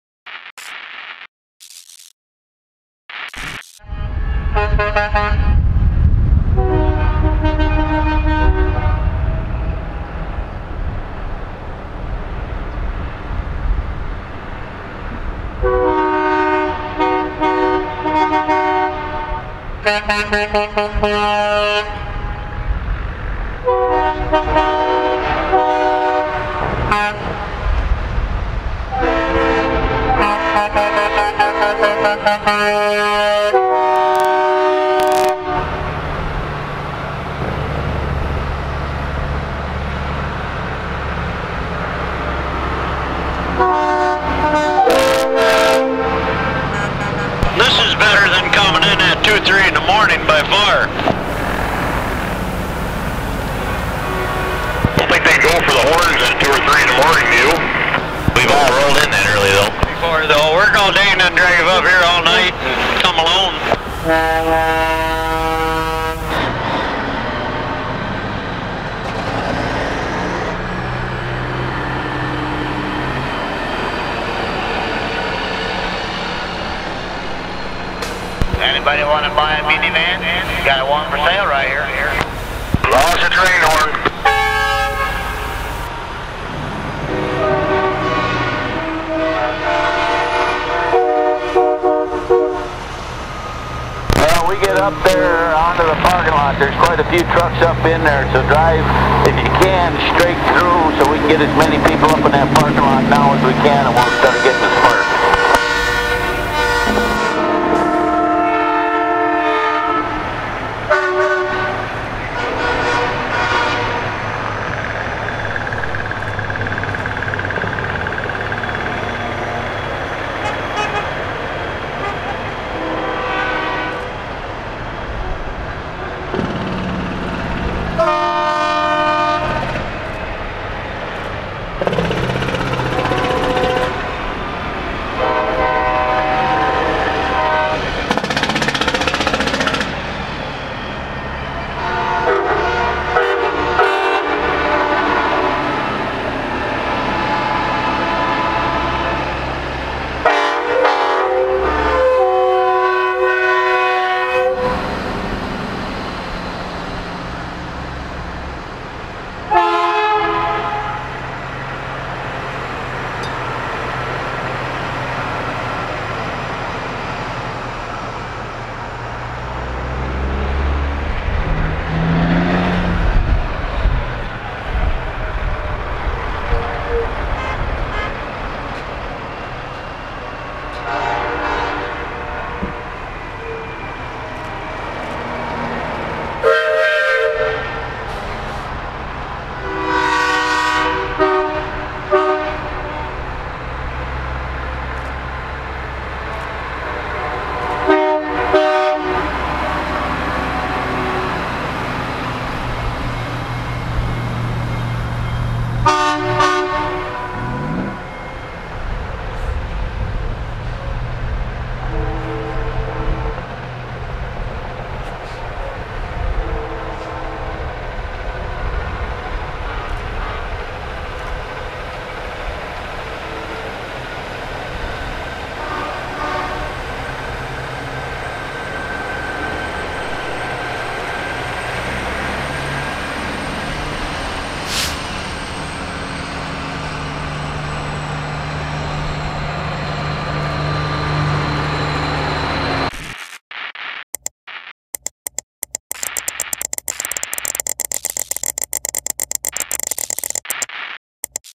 Street Petes Convoy to St.Ignace, Michigan
One of many cool shots on the 2013 Richard Crane Memorial Truck Show DVD available at 4 State Trucks.

Produced By: Christopher E. Fiffie